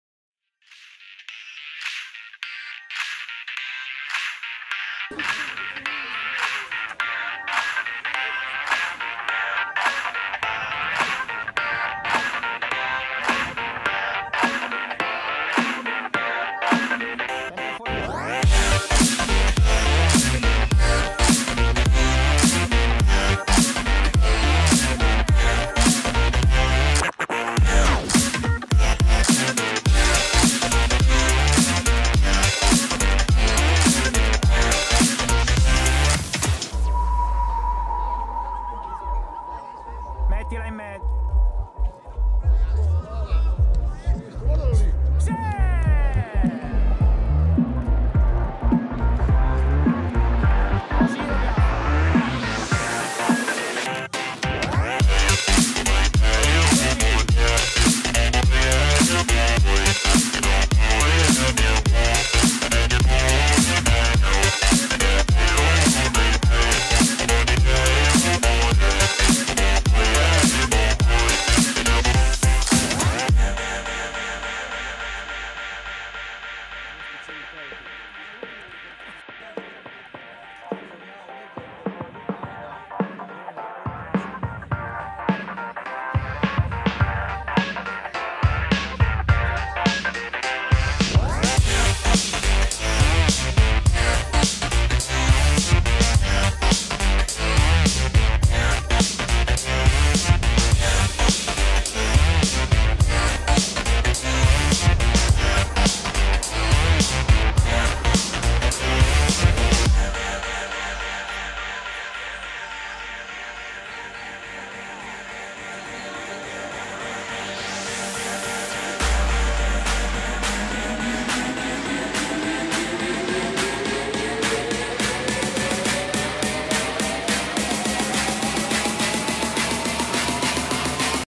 Busca vs Saluzzo
Torneo di Saluzzo 2019
Risultato finale Busca 3 - Saluzzo 1